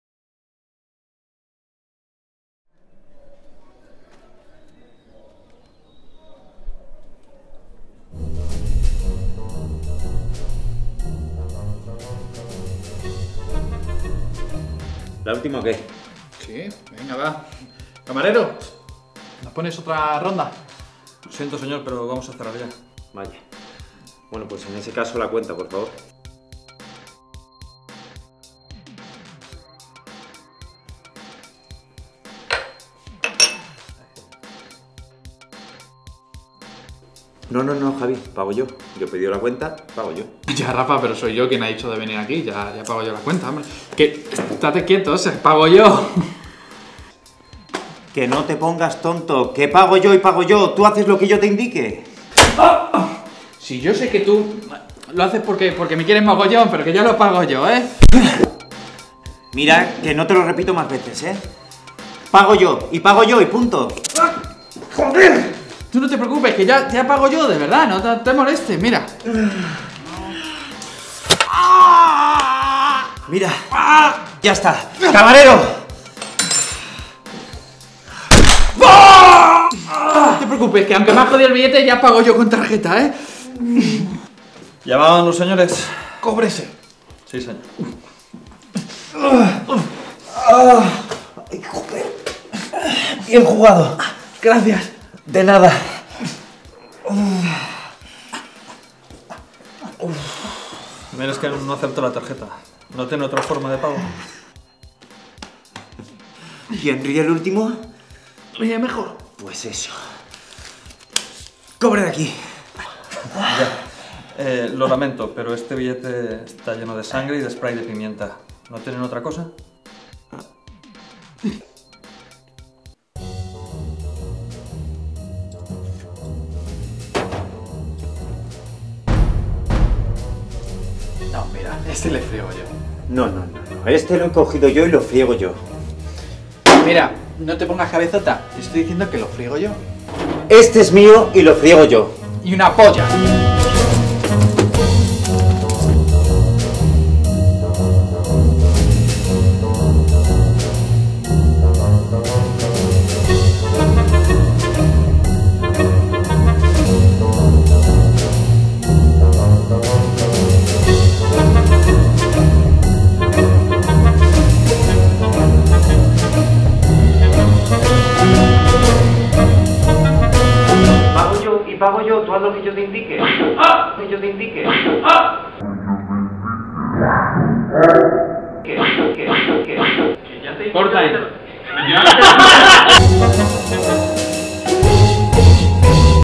Pago yo cortometraje short film
Rodado en cine con Blackmagic cinema camera ópticas: Rokinon cine f/1.5 35mm, 85mm y 14mm.Iluminación led.